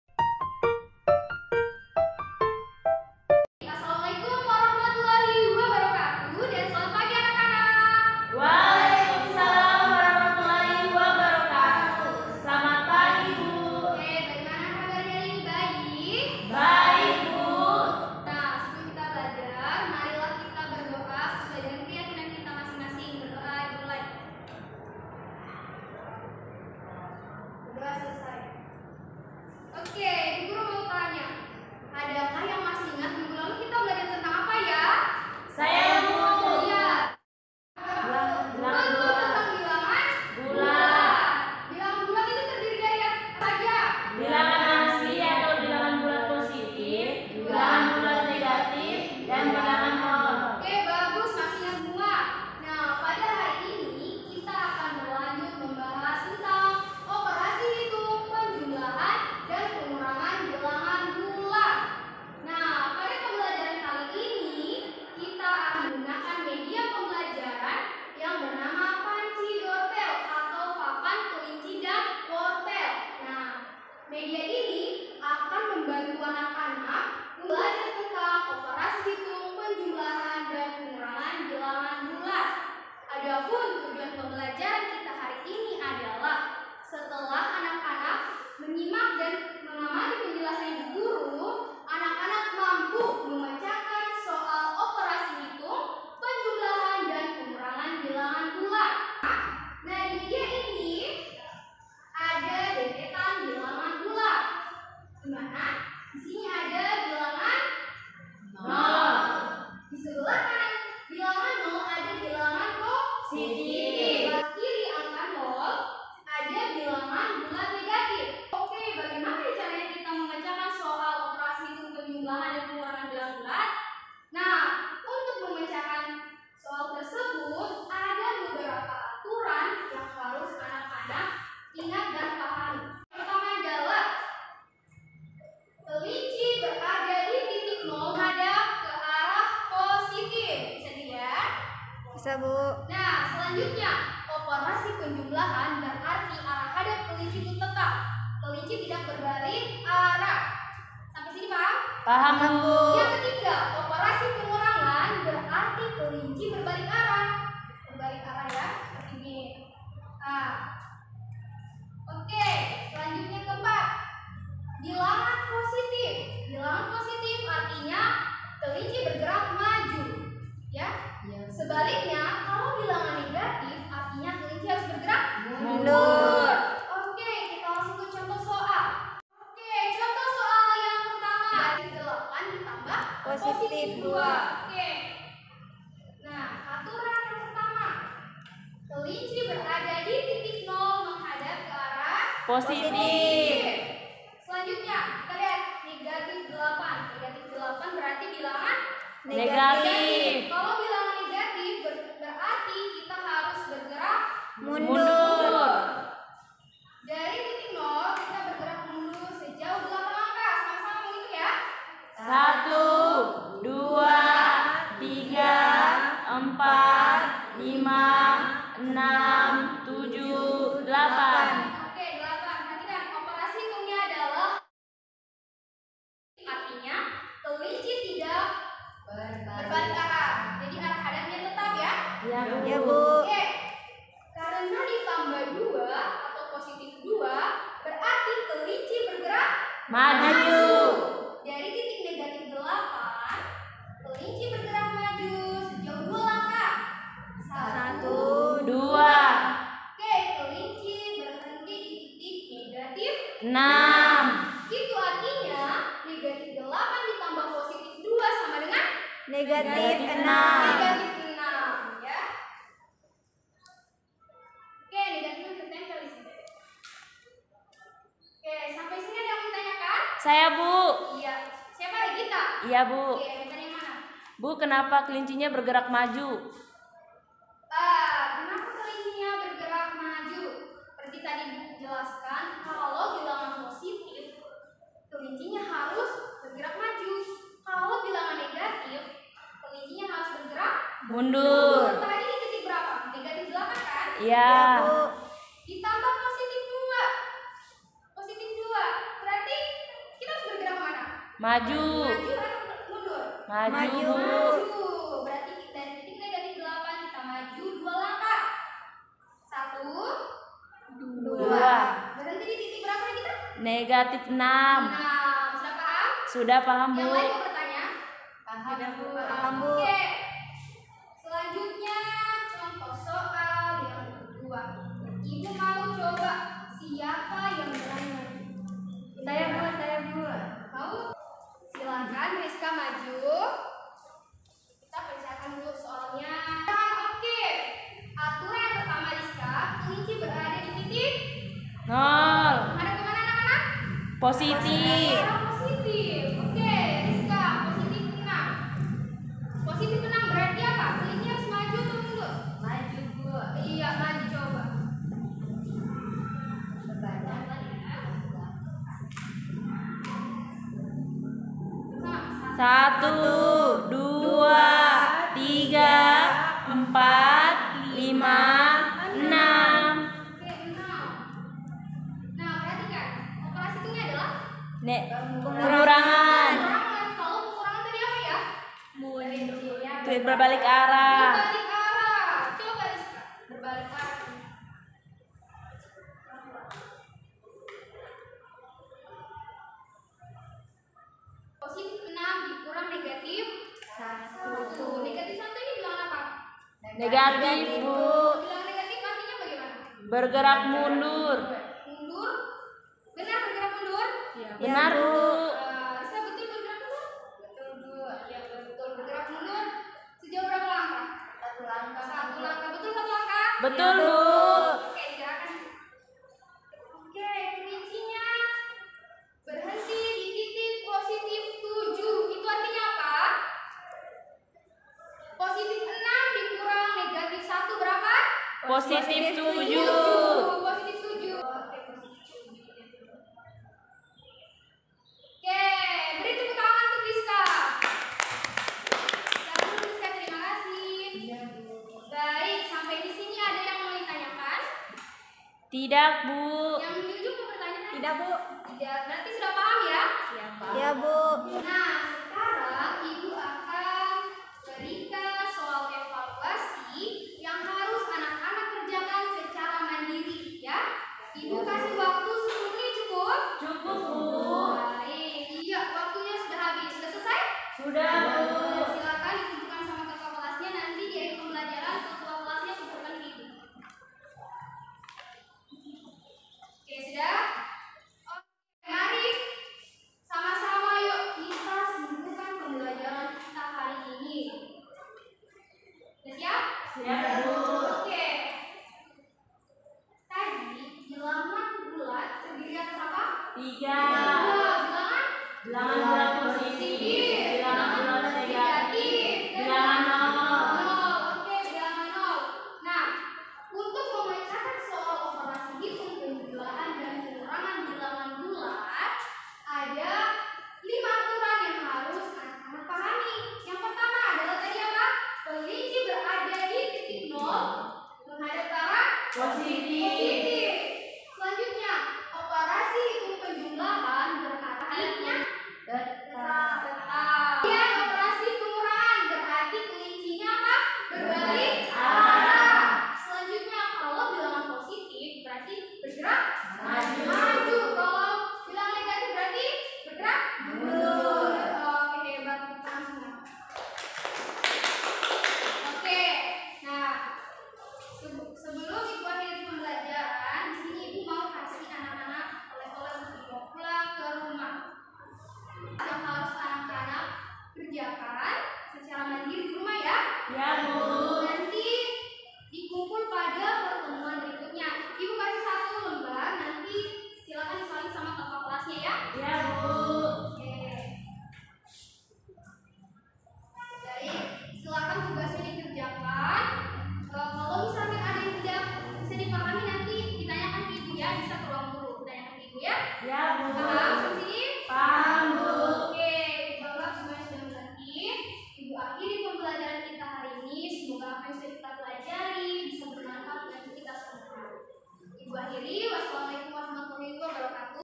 08 Microteaching - Dilla Herliana
Lomba Microteaching/Lomba Mengajar Kreatif di Era Kenormalan Baru/Operasi Hitung Penjumlahan dan Pengurangan Bilangan Bulat Menggunakan Media Pancidortel

Nama : DILLA HERLIANA (A1G120006)
Prodi  : Pendidikan Guru Sekolah Dasar (PGSD) S1, Fakultas Keguruan Dan Ilmu Pendidikan, Universitas Halu Oleo

Materi/Pokok Bahasan: Operasi Hitung Penjumlahan Dan Pengurangan Bilangan Bulat.

Kompetensi Dasar:
3.2.Menjelaskan dan melakukan operasi penjumlahan, pengurangan, perkalian dan pembagian yang melibatkan bilangan bulat negatif

Indikator Pencapaian Kompetensi Dasar
3.2.1. Memecahkan soal operasi hitung penjumlahan bilangan bulat
3.2.2. Memecahkan soal operasi hitung pengurangan bilangan bulat.